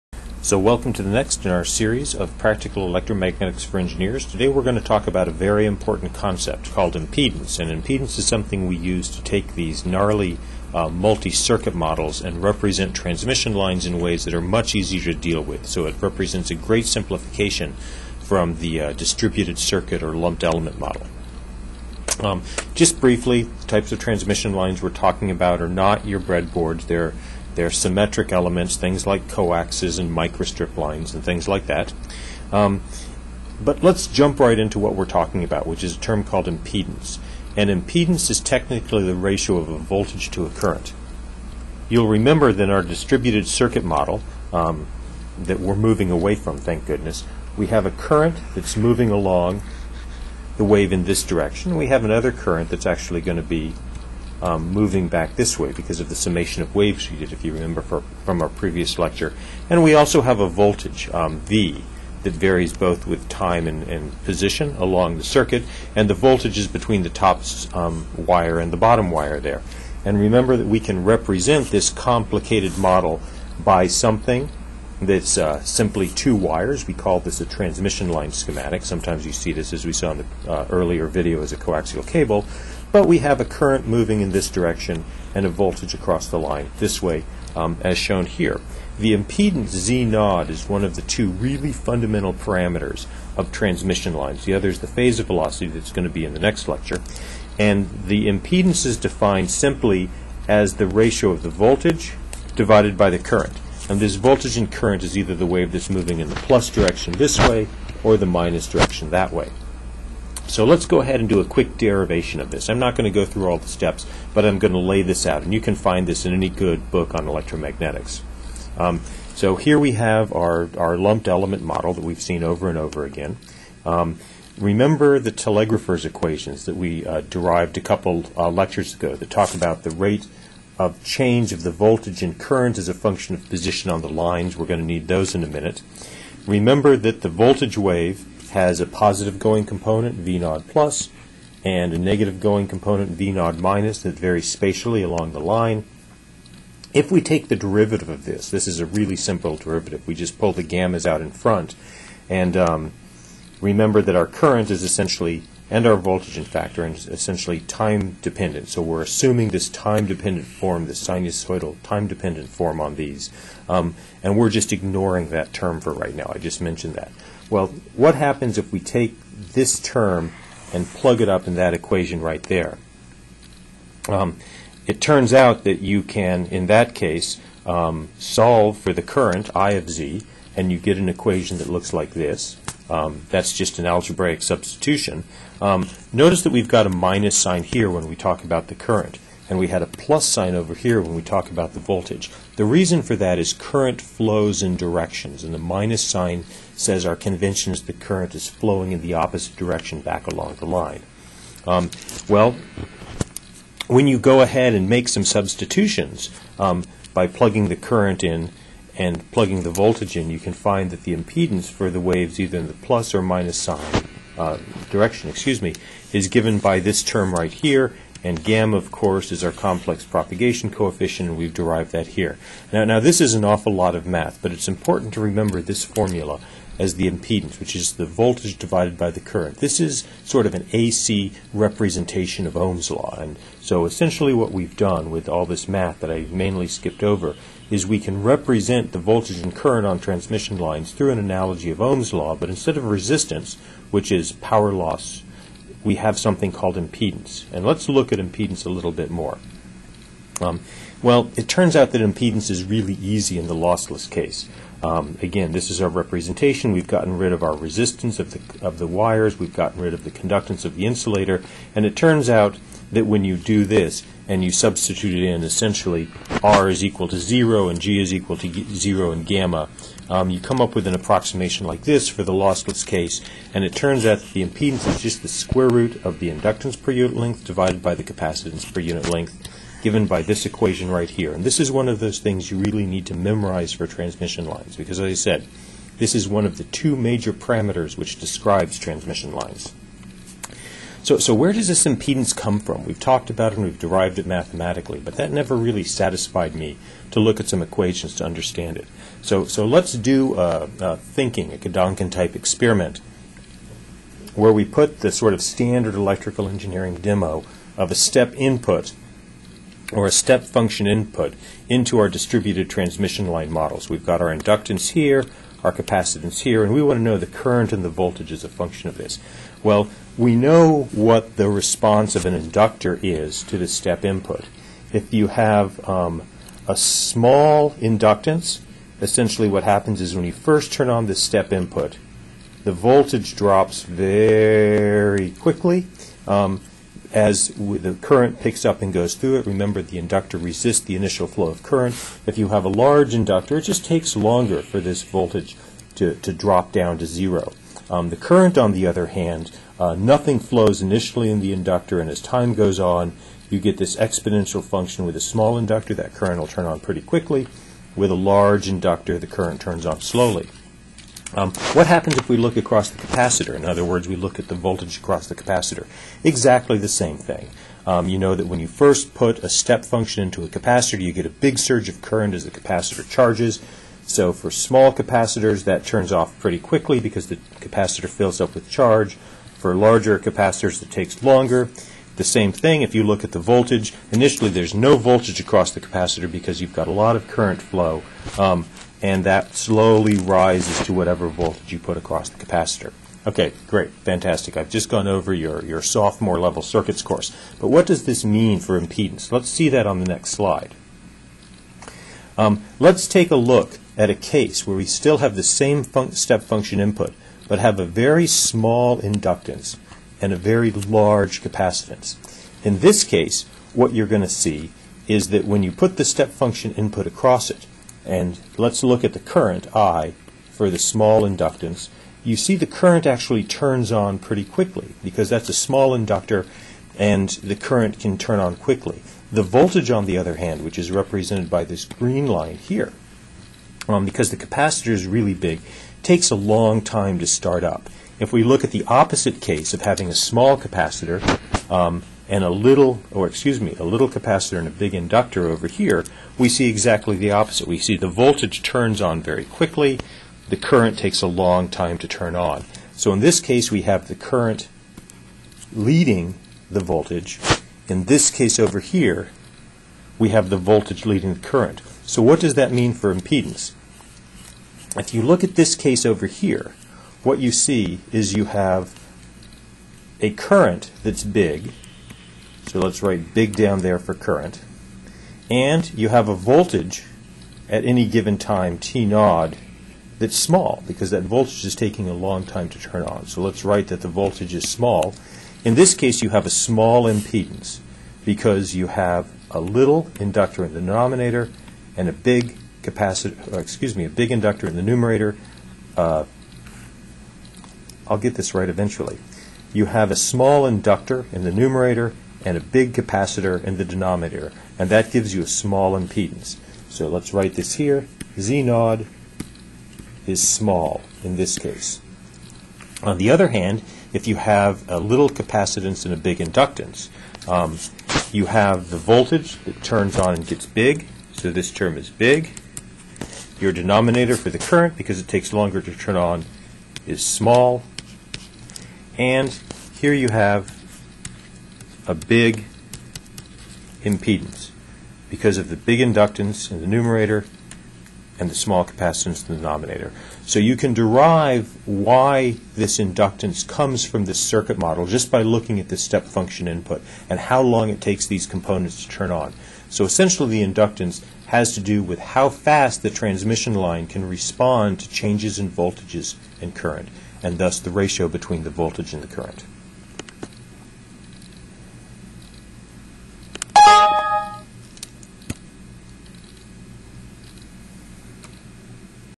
1.3 Impedance
This video was made for a junior electromagnetics course in electrical engineering at Bucknell University, USA.  The video is designed to be used as the out-of-the-classroom component and combined with active learning exercises in class.  This video covers the concept of impedance on a transmission line